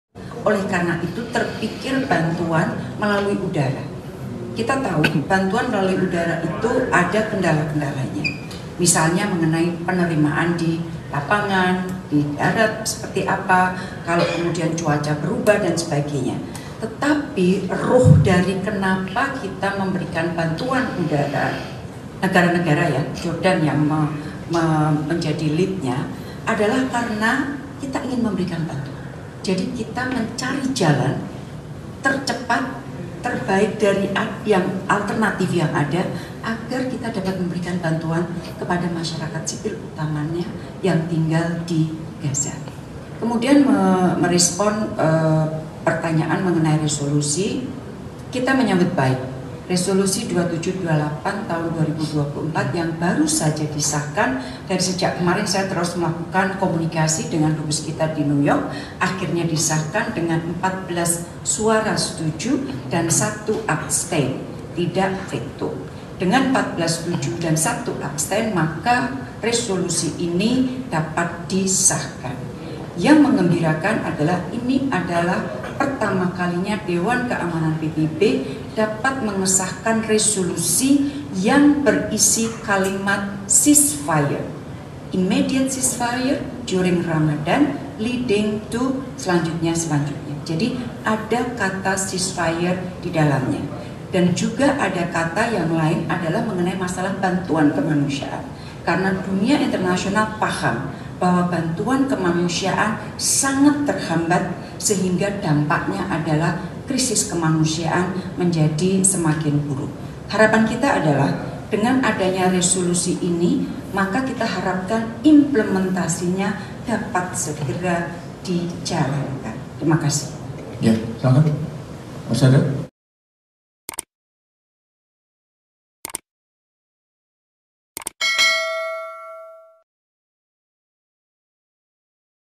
Menlu: Menggembirakan, DK PBB Sahkan Resolusi Gencatan Senjata di Gaza
Pemerintah Indonesia menyambut baik resolusi gencatan senjata di Gaza, Palestina, disetujui oleh Dewan Keamanan Perserikatan Bangsa-Bangsa (DK PBB).

Menteri Luar Negeri RI Retno Marsudi mengatakan, pertama kalinya DK PBB mengesahkan resolusi berisi kalimat "ceasefire" atau gencatan senjata.

Selain itu, kata Retno, dalam resolusi DK PBB disinggung pula bantuan kemanusiaan bagi warga Palestina di Gaza.

Retno berharap resolusi yang telah disahkan oleh PBB segera dilaksanakan.

Video Jurnalis: Pramulya Sadewa
Penulis Naskah: Pramulya Sadewa
Video Editor: Pramulya Sadewa
Produser: Nursita Sari